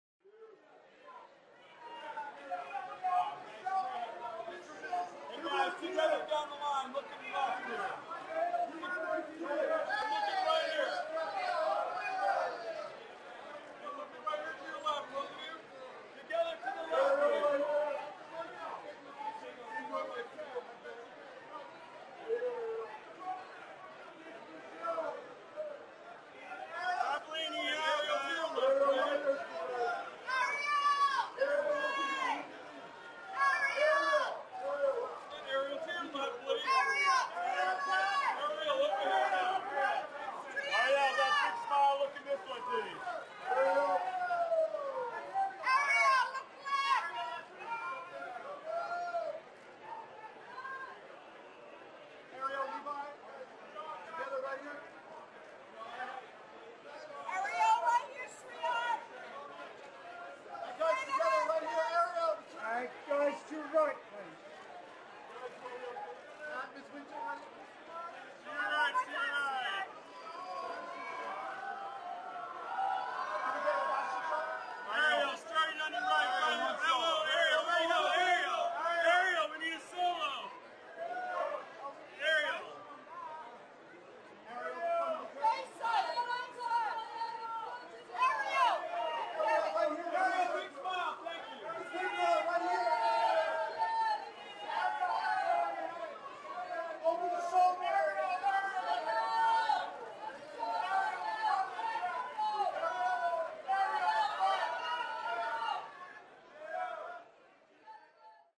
Ariel Winter mania 2017 | beau Levi Meaden and Emmys | Ariel Winter arrives | HD 720p
Emmy Awards with boyfriend Levi Meaden - Ariel Winter mania 2017 -
daring double thigh-split gown - Ariel put on a leggy display in a daring double

This video shows Ariel Winter mania 2017 - beau Levi Meaden and Emmys